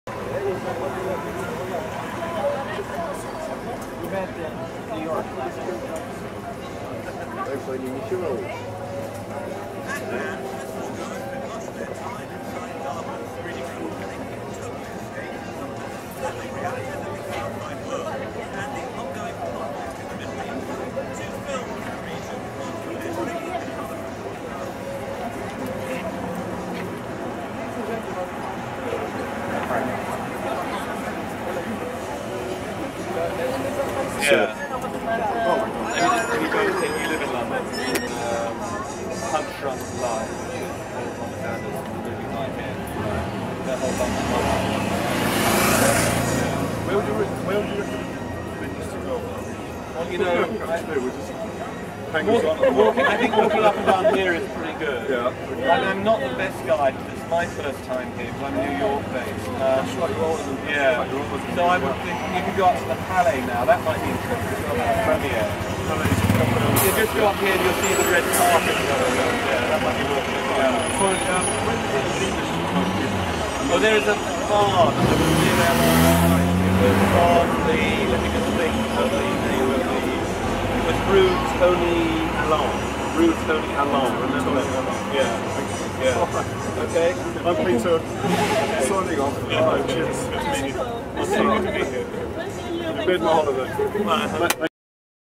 Meeting Tom Brook in Cannes, France - 2002
Meeting Tom Brook from the BBC show TALKING MOVIES at the Cannes film festival 2002, his camerawomen actually held my camera while I spoke to him. Top man!